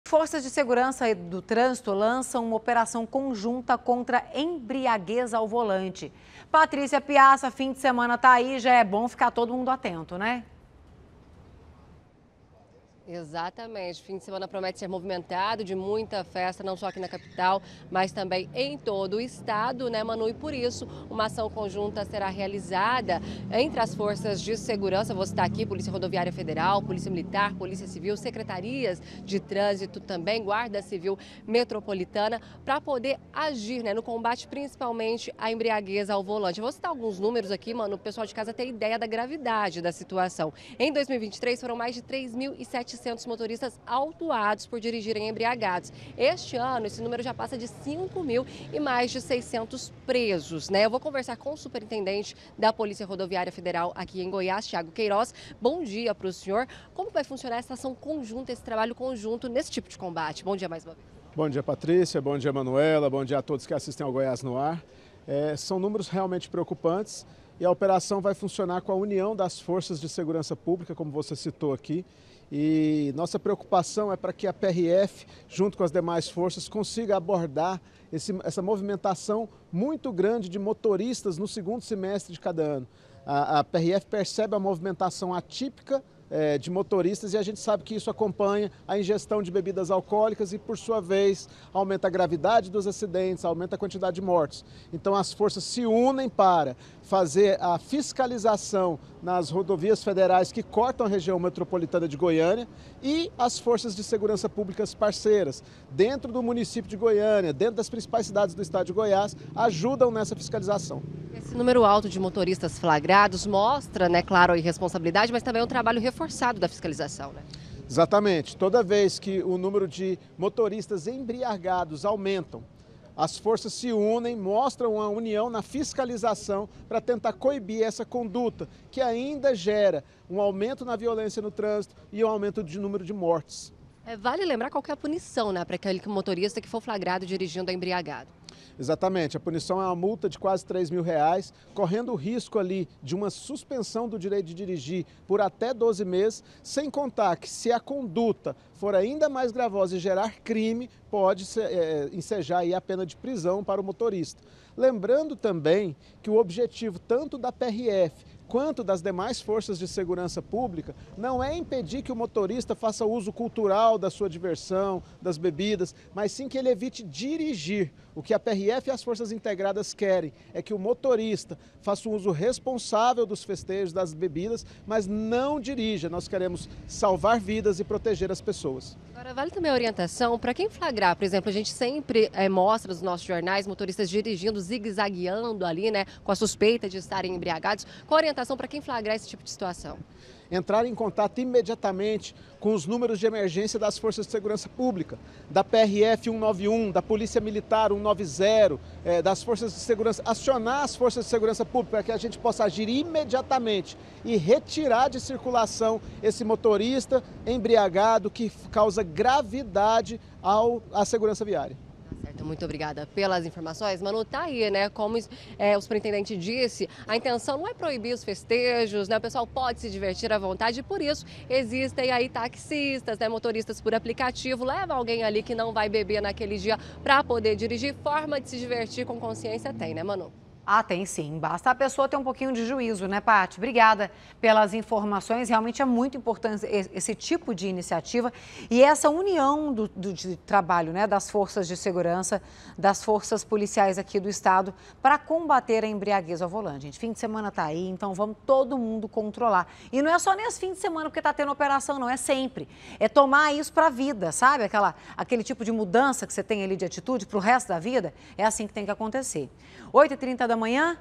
EMBRIAGUEZ AO VOLANTE
FORÇAS DE SEGURANÇA E DE TRÂNSITO LANÇAM OPERAÇÃO CONJUNTA CONTRA EMBRIAGUEZ AO VOLANTE.